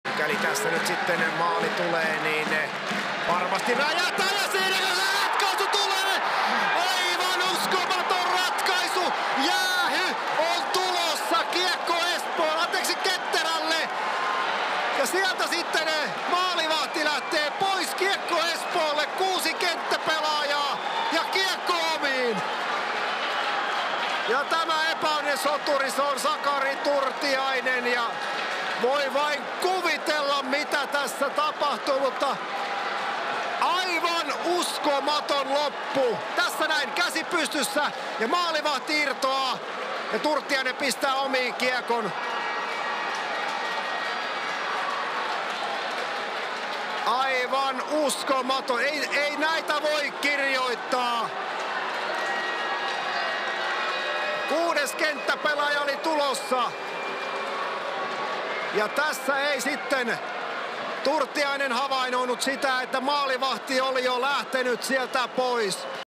Uskomaton ratkaisu toisessa jatkoerässä! 🤯
Imatran Ketterä ryösti voiton kolmannessa finaalissa Espoosta ennätysyleisön edessä!
Ottelun ratkaisu nähtiin uskomattomalla tavalla ajassa 84:30!

Kaikki Mestis-matsit näet C Moresta!

Selostus: Toni Lång